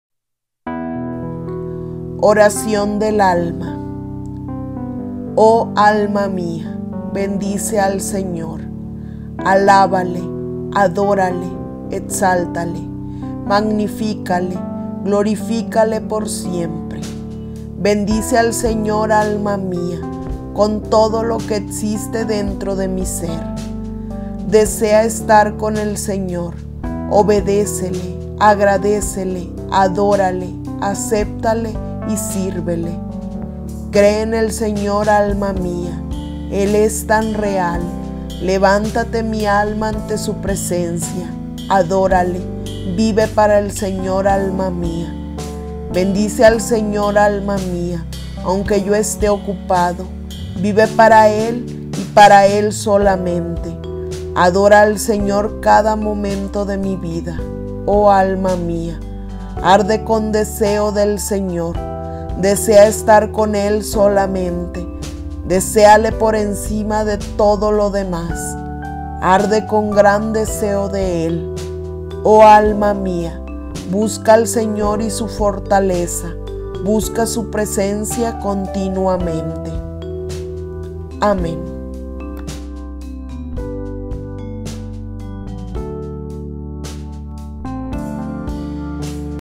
▶ ORACIÓN DEL ALMA - ORACION Y PAZ
Oremos para que nuestra alma le sirva en todo momento a Jesucristo Nuestro Señor.